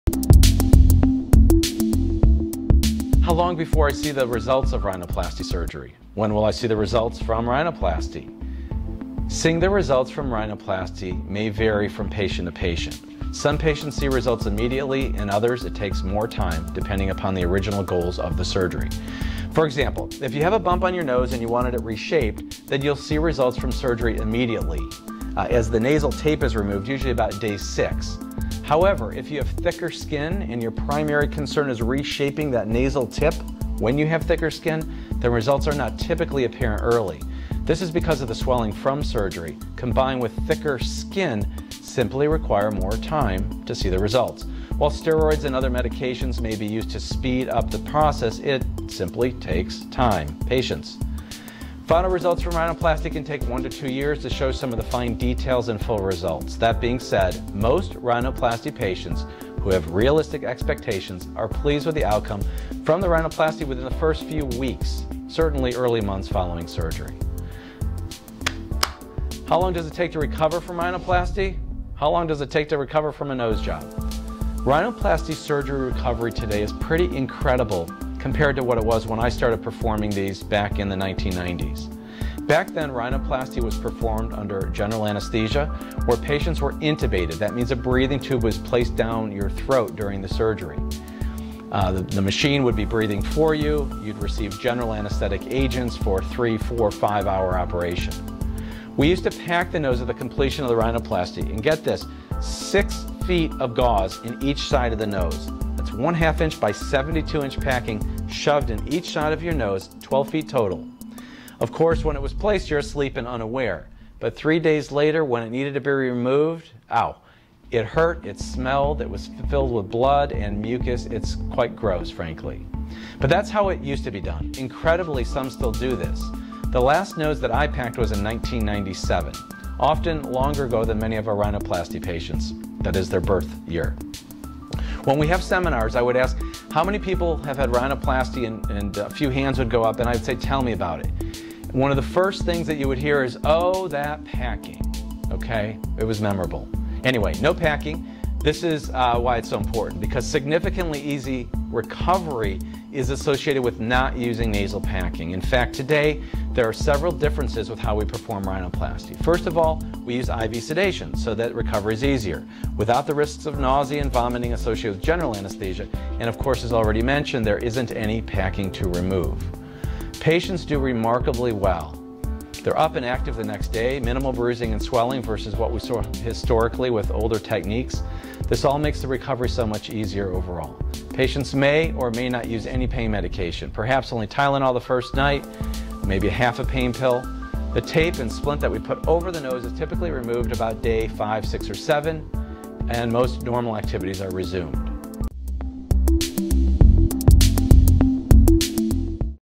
When Will I First See Results From Rhinoplasty Procedure? (Best Rhinoplasty Procedure 2020)
How long before I see the results of rhinoplasty surgery. When will I see the results from rhinoplasty. Seeing the results from rhinoplasty may vary from patient to patient.

Some patients see results immediately and others it takes more time depending upon the original goals of the surgery.

For example if you have a bump on your nose and you wanted to reshape that you'll see results from surgery immediately as the nasal tape is removed usually about Day 6. However if you have thicker skin and your primary concern is reshaping that nasal tip when you have thicker skin, he results are not typically apparent early. This is because of the swelling from surgery combined with thicker skin simply require more time to see the results. While steroids and other medications may be used to speed up the process. It simply takes time.

Patience.  Final results for rhinoplasty can take 1 to 2 years to show some of the fine details and full results. That being said most rhinoplasty patients who have realistic expectations are pleased with the outcome from the rhinoplasty within the first few weeks certainly early months following surgery. How long does it take to recover from rhinoplasty.

How long does it take to recover from a nose job rhinoplasty surgery? Recovery today is pretty incredible compared to what it was when I started performing these back in the 1990s. Back then rhinoplasty was performed under general anesthesia where patients were intubated. That means a breathing tube was placed down your throat during the surgery the machine would be breathing for you. You'd received general anesthetic agents for three four five hour operation. We used to pack the nose on the completion of the rhinoplasty and get this, six feet of gauze in each side of the nose.

That's one half inch by 72 inch packing shoved in each side of your nose.

Twelve feet total. Of course when it was placed your sleeping, unaware. But three days later when it needed to be removed, ouch, it hurt, it smelled.

It was filled with blood and mucus. It's quite gross frankly. But that's how it used to be done.

Incredibly some still do this. The last nose that I packed was in 1997, often longer ago than many of our rhinoplasty patients.

That is their birth year. When we have seminars I would ask how many people have had rhinoplasty and a few hands would go up and I would say "Tell me about it." One of the first things that you would hear is Ohhhh! It was memorable anyway. No packing. This is why it's so important because significantly easy recovery is associated with not using nasal packing in fact today, there are several differences with how we perform rhinoplasty. First of all, we use I.V. sedation so that recovery is easier, without the risks of nausea and vomiting associated general anesthesia.

And of course as already mentioned there isn't any packing to remove. Patients do remarkably well. They're up and active the next day. Minimal bruising and swelling versus what we saw historically with older techniques. This all makes the recovery so much easier.

Overall patients may or may not use any pain medication perhaps only Tylenol the first night. Maybe a half a pain pill. The tape and splint that we put over the nose is typically removed about day five six or seven and most normal activities are resumed.